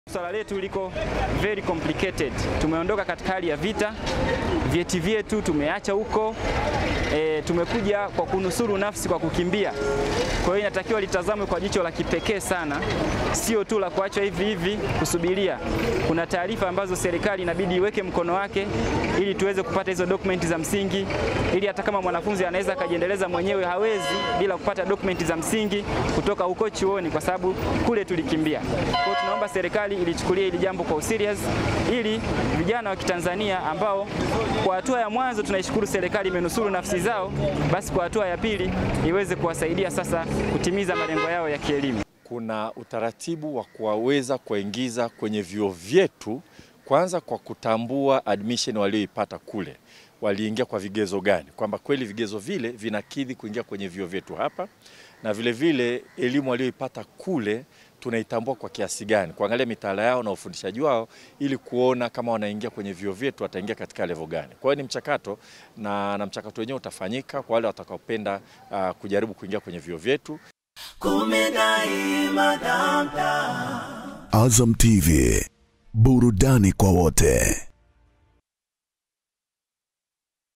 Wanafunzi waliokimbia mapigano Sudan waiangukia Serikali 'hatma yao ya elimu'
Watanzania waliokimbia mapigano nchini Sudan wametolewa hofu juu ya mustakabali wa elimu yao wakiambiwa kuwa utaratibu utawekwa na serikali kwa wanafunzi hao kuendelea na masomo hapa nchini.